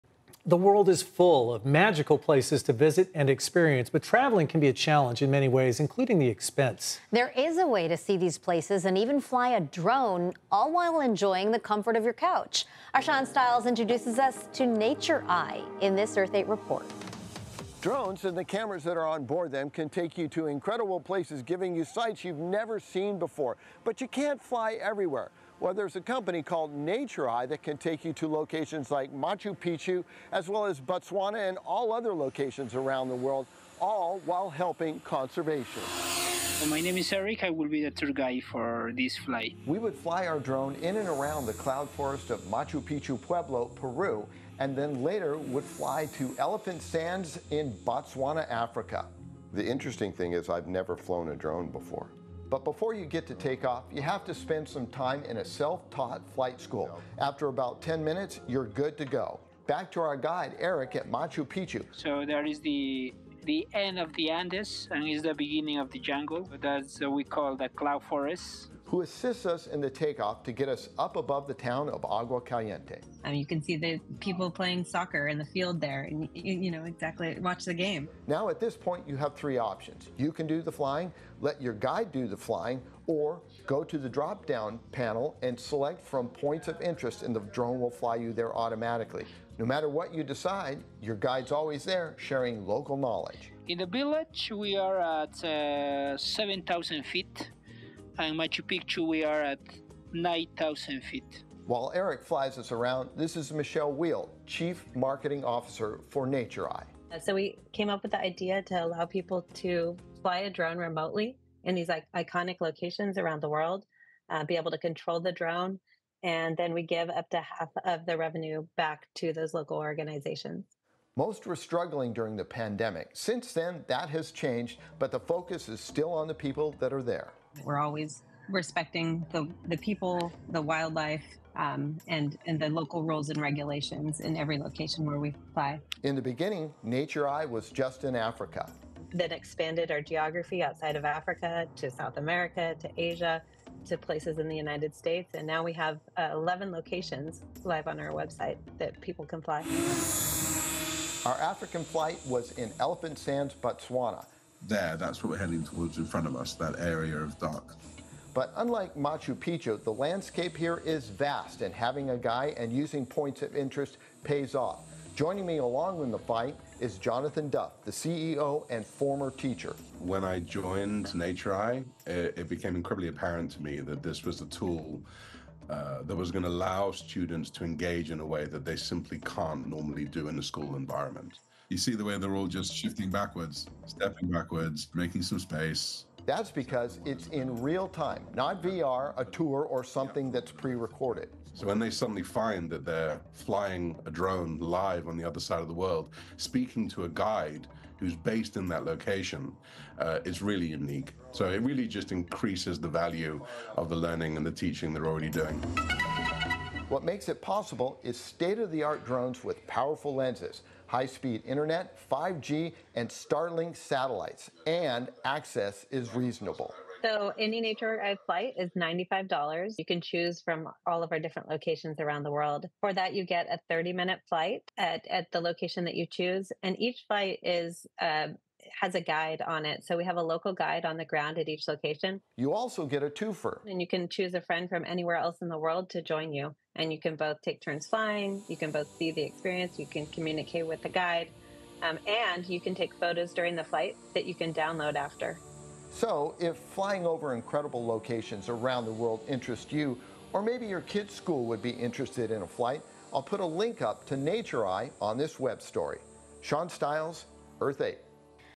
How NatureEye explores the world with high-tech remote drones
NatureEye allows you to see the world in revolutionary ways without you having to leave your home.